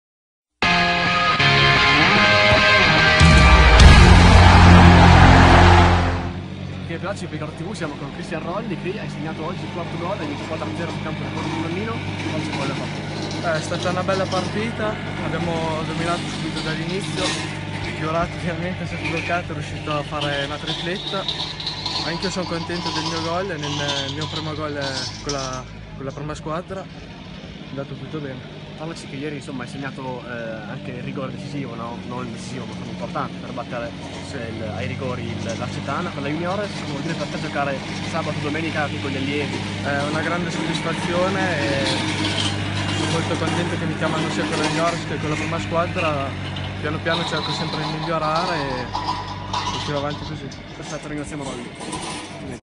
Cristian Rolli dopo partita con Borgo San Donnino. 26/03/2017
Cristian Rolli dopo partita Borgo San Donnino - Piccardo Traversetolo 0-4. 30^ Giornata Campionato di Promozione girone A. 26/03/2017.